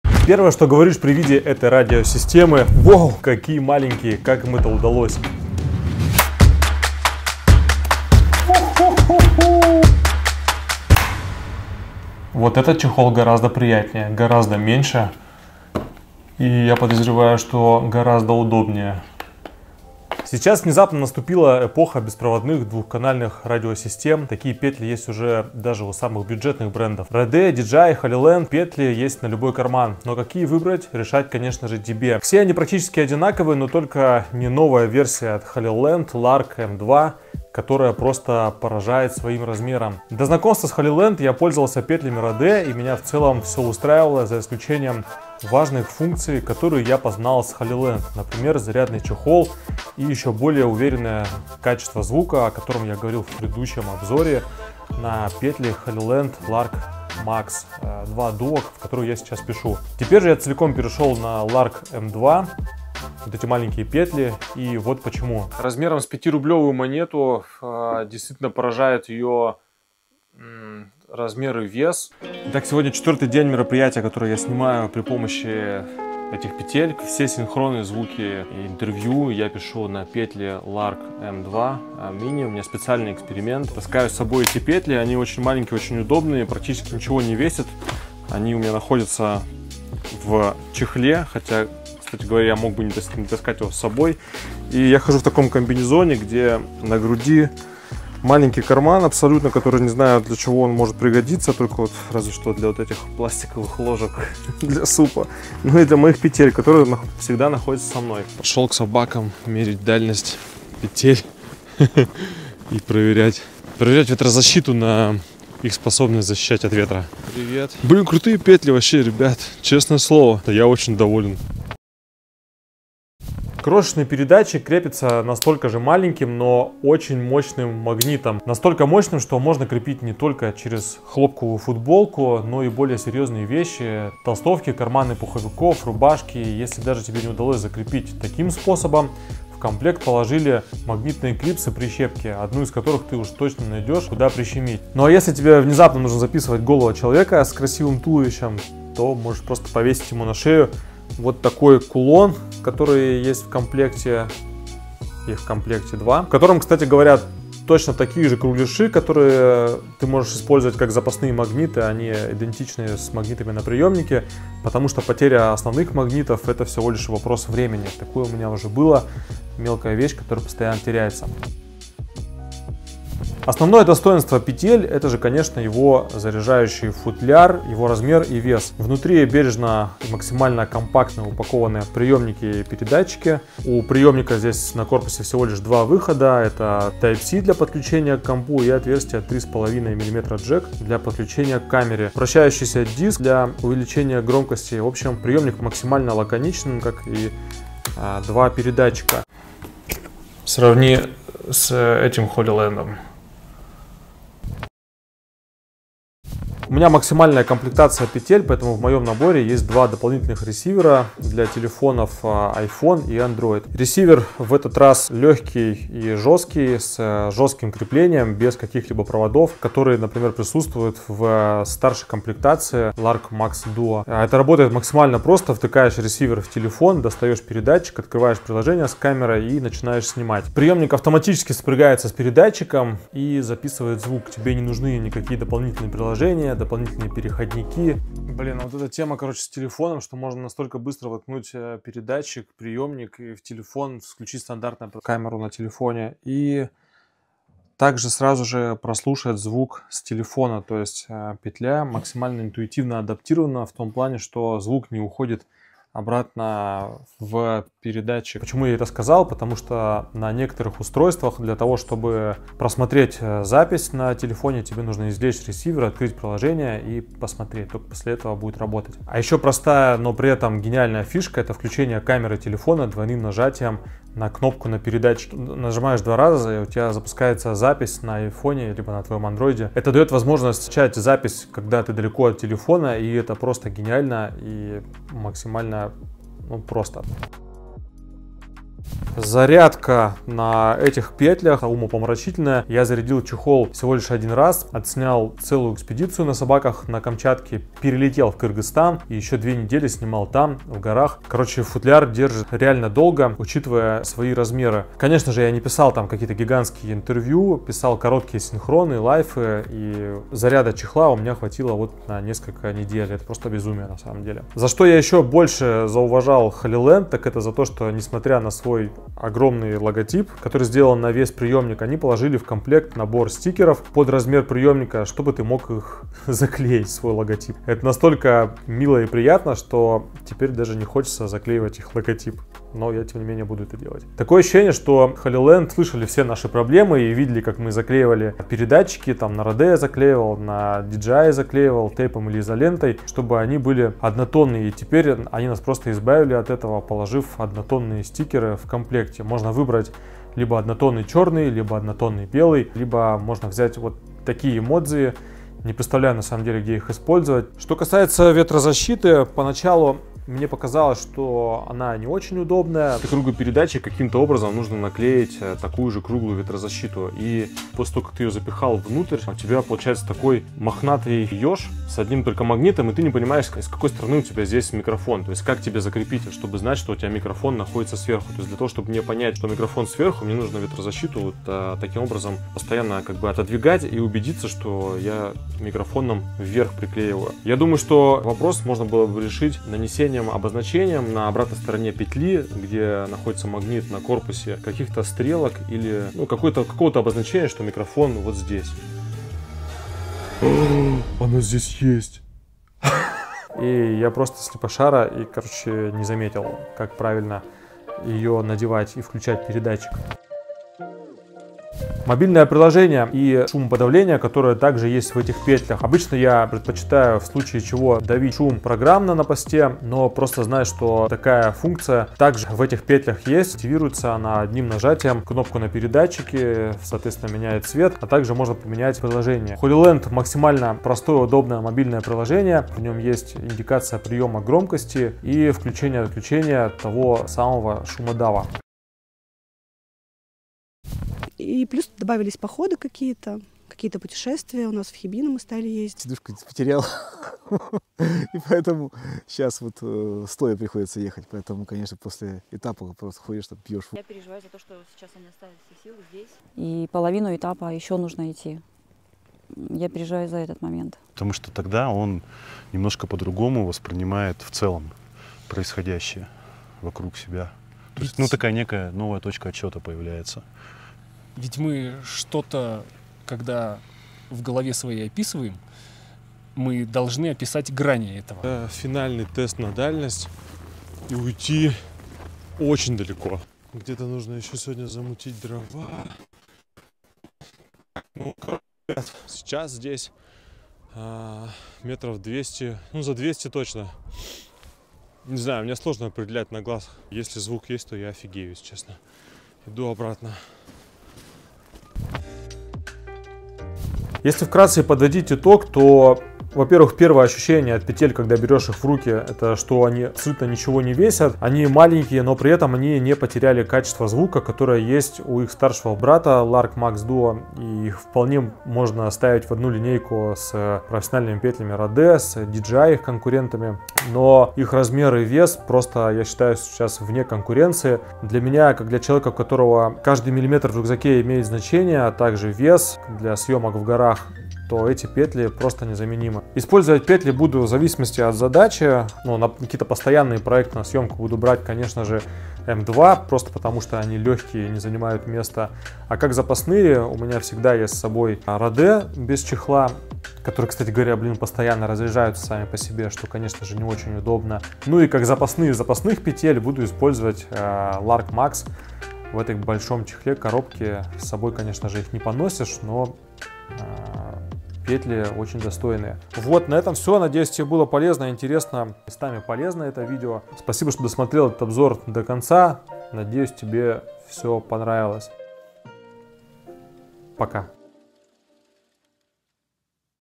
пишу звук в HOLYLAND M2 самые маленькие петли в мире. Ушел с RODE на LARK M2
Наконец-то появился внятный конкурент вездесущим радиопетлям Rode, это всем изестные Holyland  и теперь они взялись за радиосистемы, а это значит что у нас появится качественный звук за адекватные деньги.